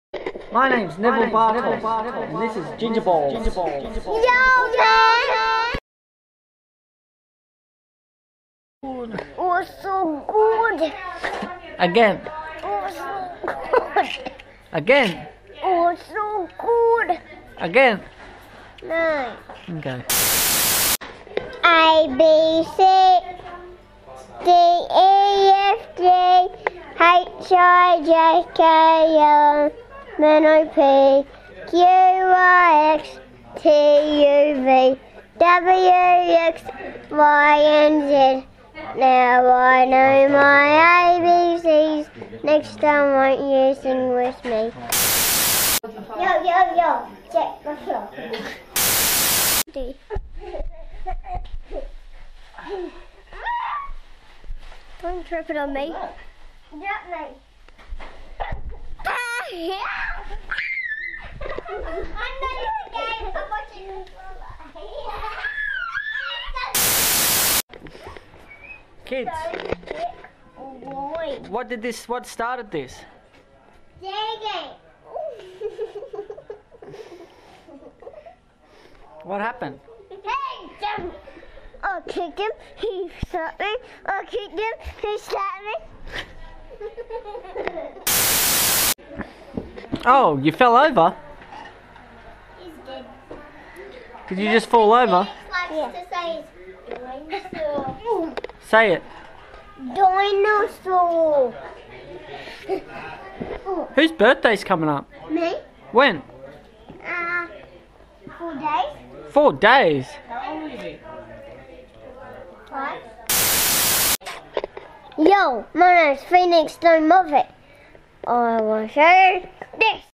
HAPPY BIRTHDAY PHEONIX! (The Ginger Nephew)
i made this video for my nephews birthday!!!

hope he likes it =)

Make Sure You Watch It In 1080p HD